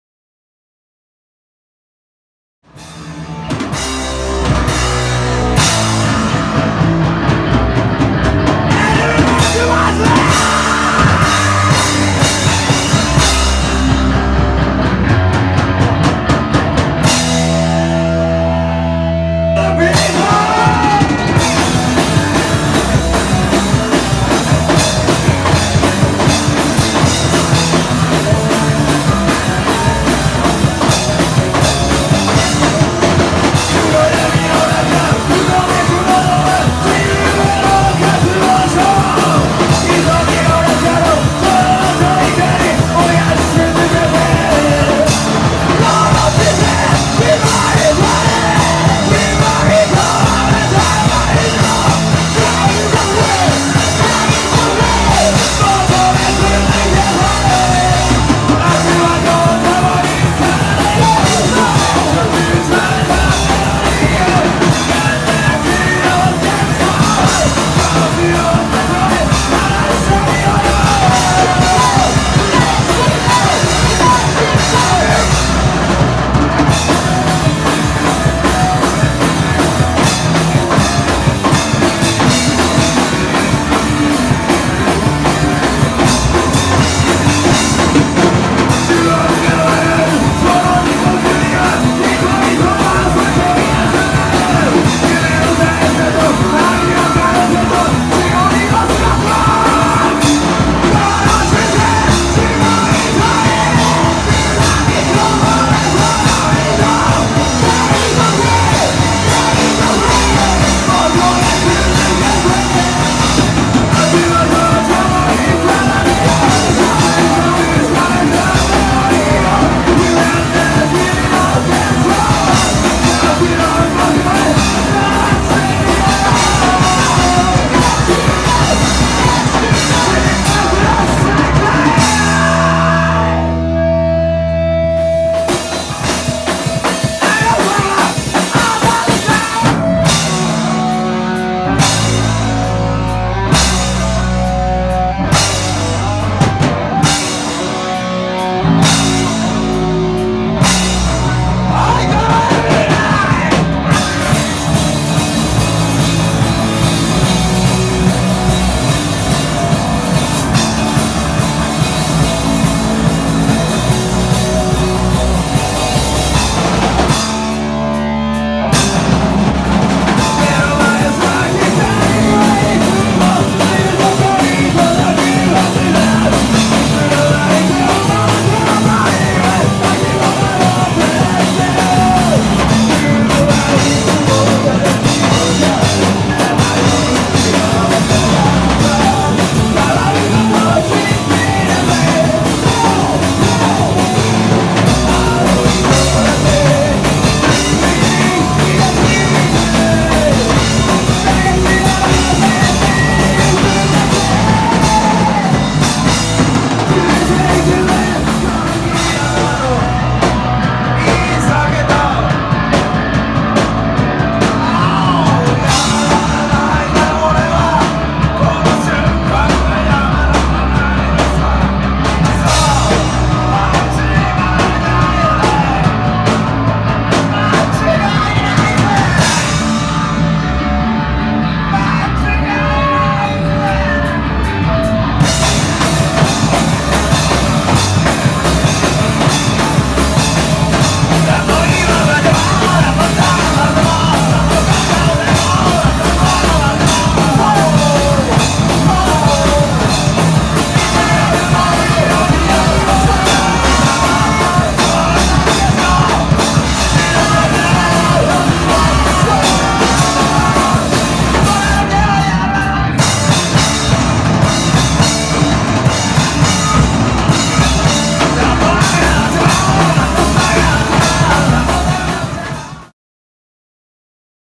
三日月爆音団 at 【ALL POWER TO THE PEOPLE vol.1】西横浜ELPUENTE 2022/7/9(sat)
2022/7/9(sat)
LIBERATION FROM SLAVES企画
【ALL POWER TO THE PEOPLE vol.1】
西横浜ELPUENTE
OPEN17:00/START18:00
ADV/DOOR　￥1500＋1DRINK
ACT　ザ☆ペラーズ
　　　BACARA
　　　三日月爆音団
　　　LIBERATION FROM SLAVES
DJ　肉神/ワタル(iROCK)
FOOD gypsy lunch box

※公開許可済み